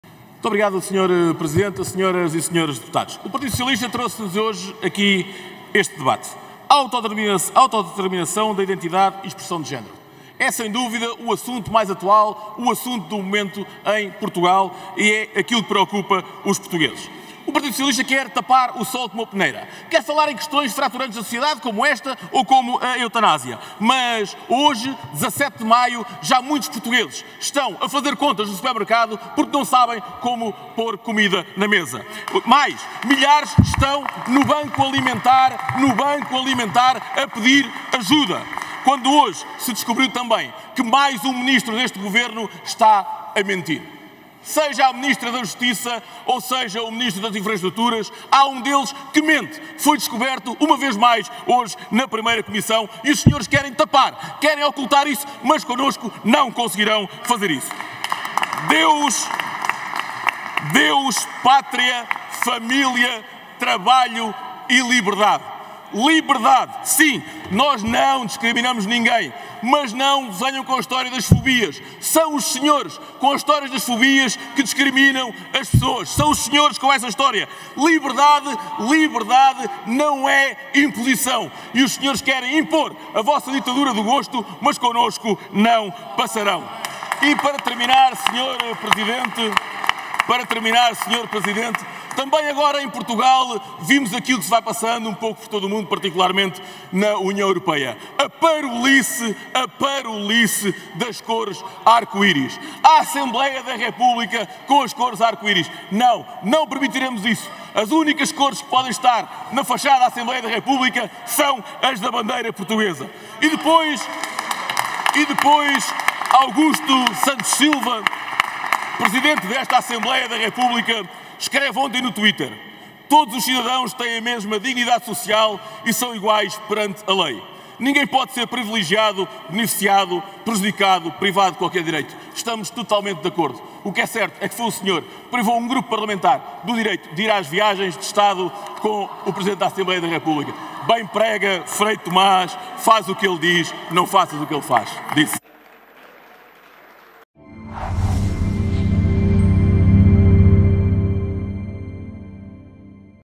Ministra da Justiça ou João Galamba! O que é certo é que um deles está a mentir
Intervenção do Líder Parlamentar do CHEGA, Pedro Pinto, no debate sobre a autodeterminação da identidade e expressão de género.